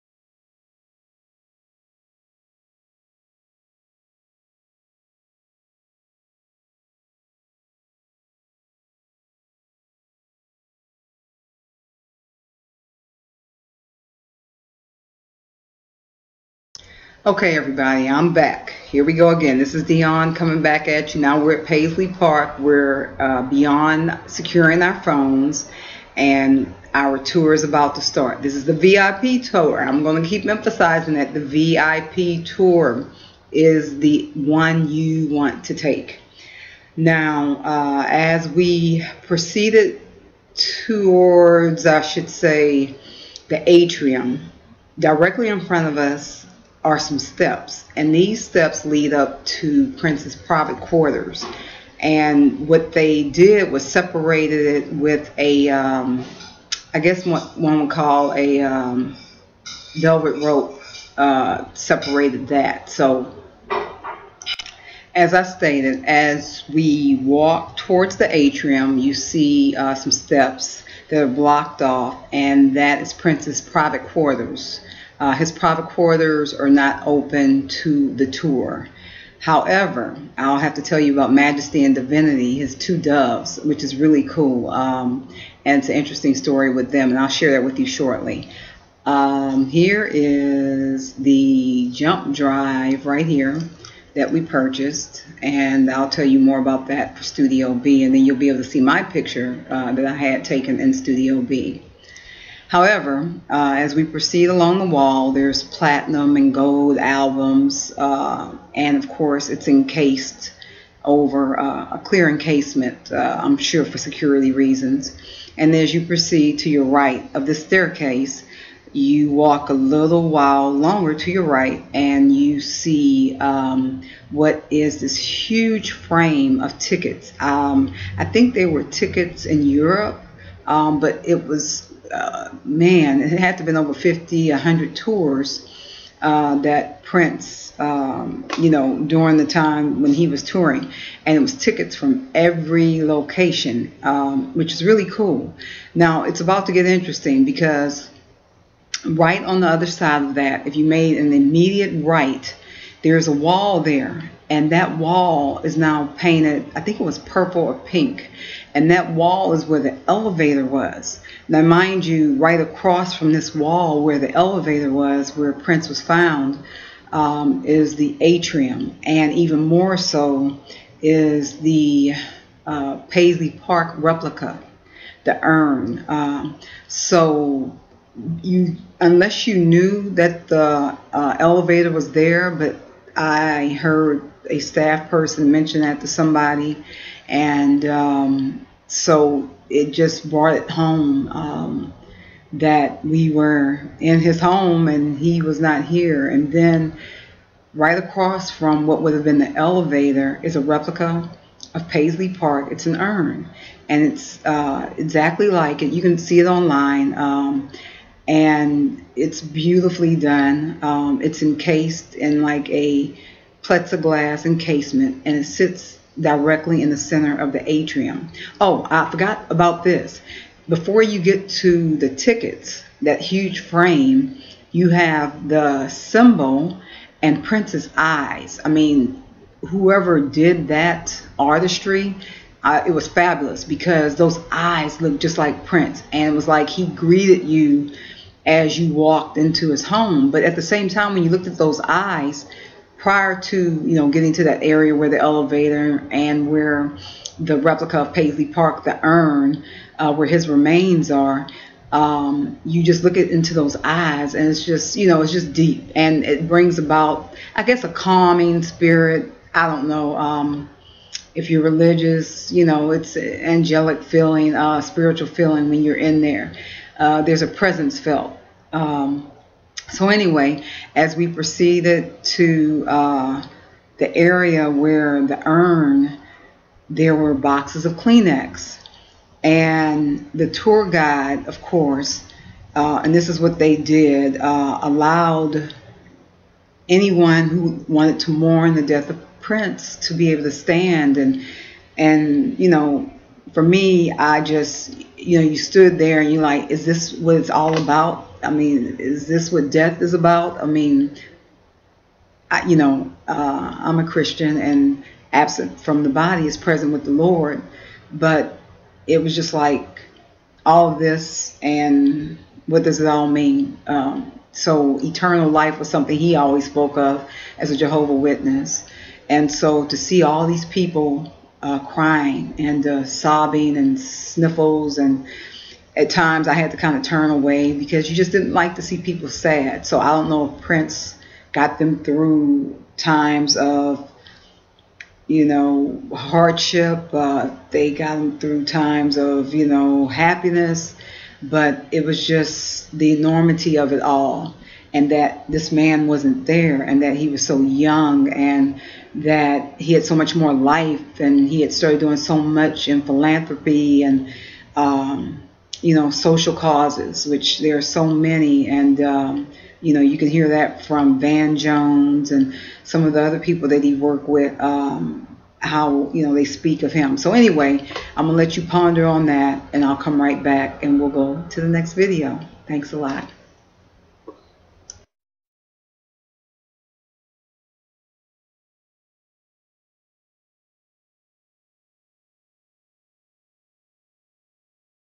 Dionne Beauregard-MY PAISLEY PARK MUSEUM EXPERIENCE - Video #5
Similar to many of you, April 21, 2016 is a day that will never be forgotten. Musical genius Prince died from an accidental overdose. After his death, I found myself reflecting on his many contributions to music, and how a wonderful life could be cut short.
In Early September 2016, I was invited to take the VIP Paisley Park Tour. With much gratitude, I accepted the invitation. I will forever be grateful for this opportunity. On October 22, 2016, I toured Paisley Park.
Unfortunately, personal video and photography were prohibited. However, for those of you who have yet to visit Paisley Park, GUESS WHAT, I have created a series of videos describing my awesome tour. UNTIL YOU ARE ABLE TO VISIT. Please allow me to be your special GUIDE. I WILL descriptively BRING PAISLEY PARK TO YOU. This is my personal perspective therefore; I invite you to enjoy the ride.